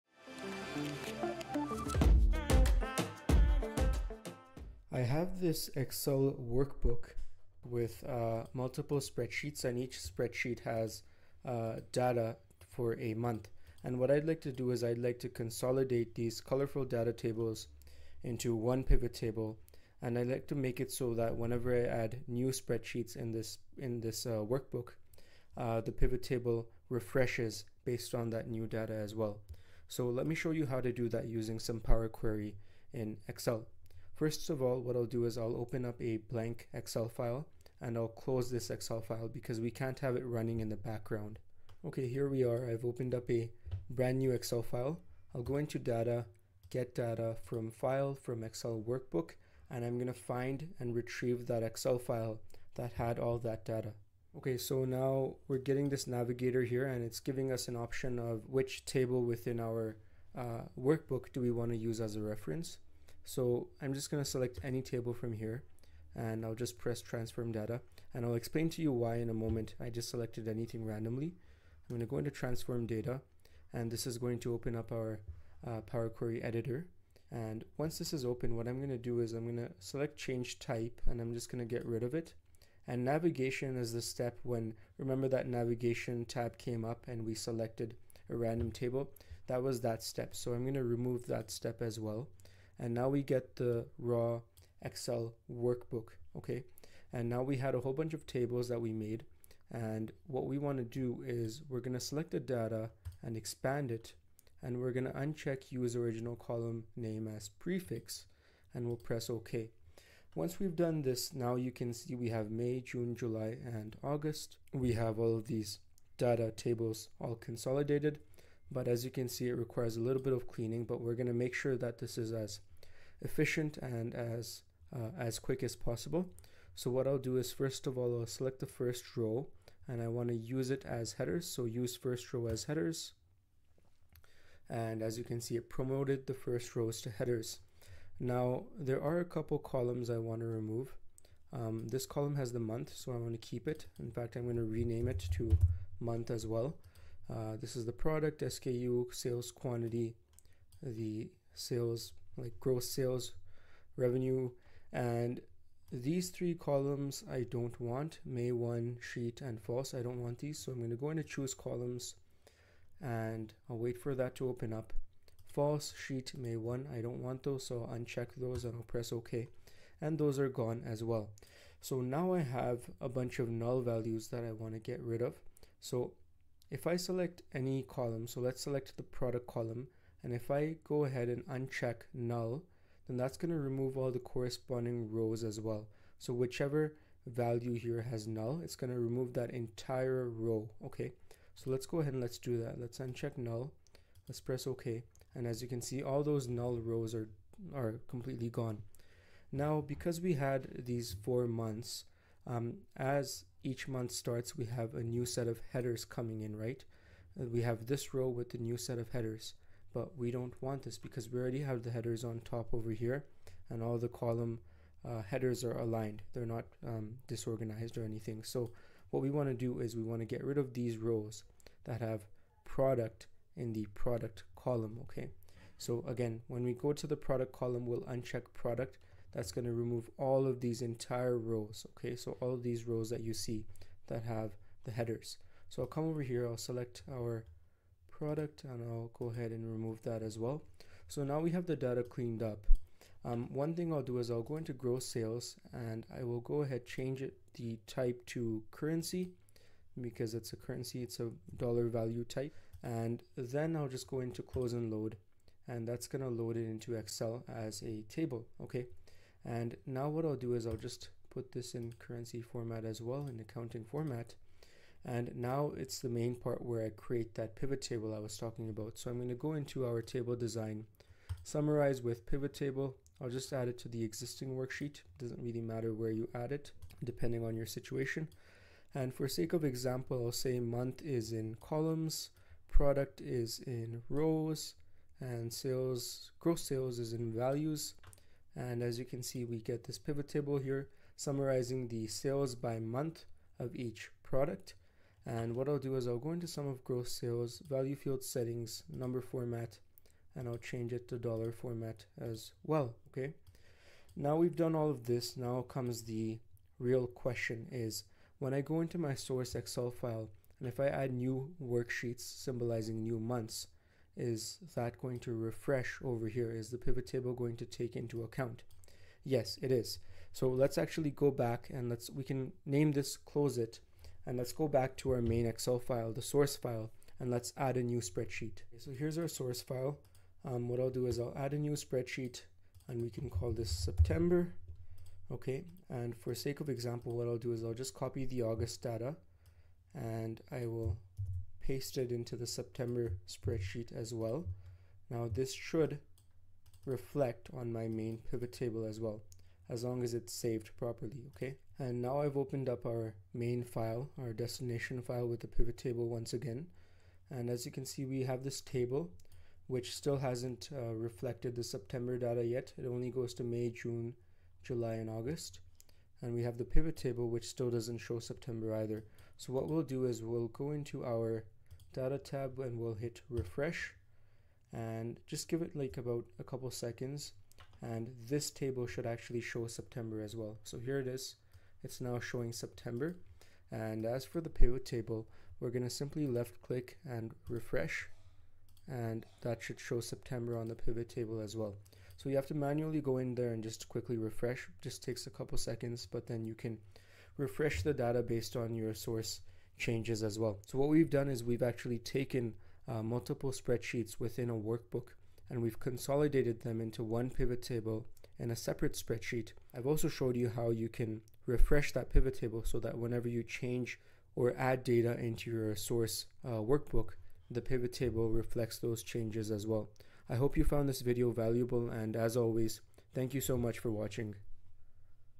How to Create a Pivot Table from Multiple Sheets in Excel!
Unlock the full potential of Excel with our comprehensive tutorial on creating PivotTables from multiple worksheets and workbooks. This tutorial is perfect for data engineers, business analysts, and anyone interested in business intelligence and data science. Master this skill and level up your data analysis game!

Whether you're a beginner or an experienced user, this step-by-step guide will demystify the process and empower you to consolidate and analyze data effortlessly. PivotTables are indispensable tools for summarizing and gaining insights from complex datasets, and in this tutorial, you'll learn how to harness their power to make informed decisions.

Join us as we walk you through each step, from combining data from different sources to customizing your PivotTables to suit your analytical needs.